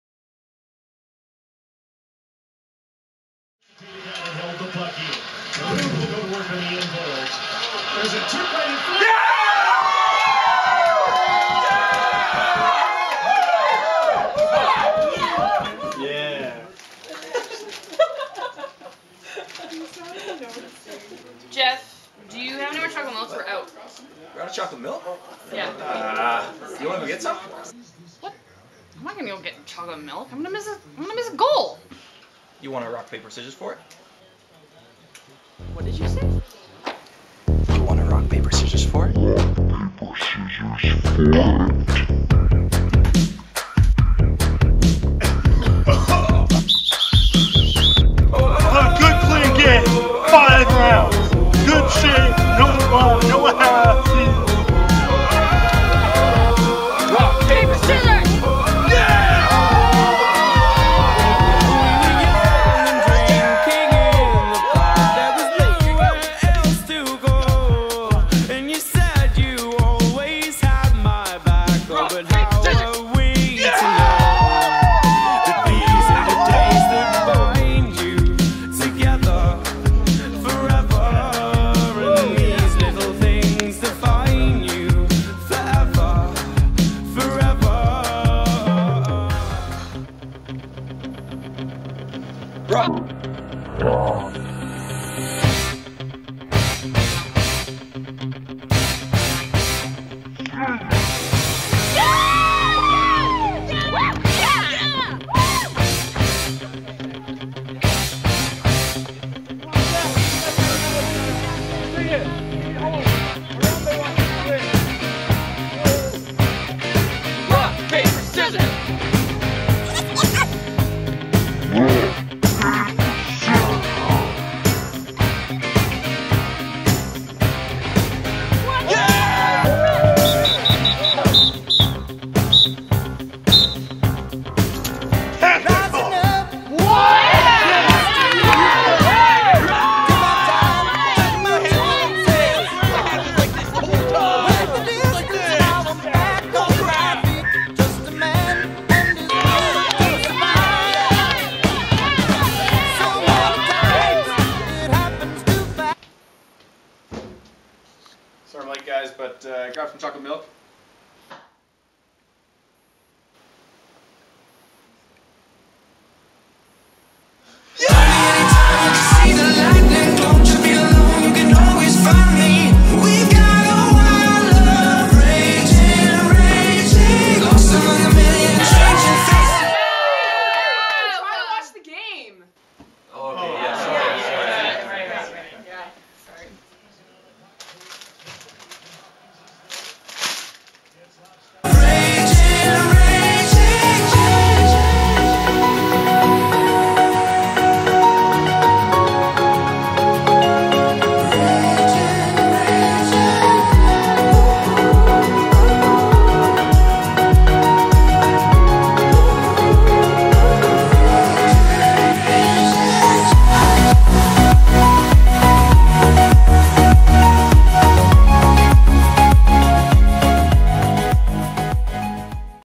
Chocolate Milk
Two friends engage in an intense round of rock-paper-scissors to see who's going to get the chocolate milk.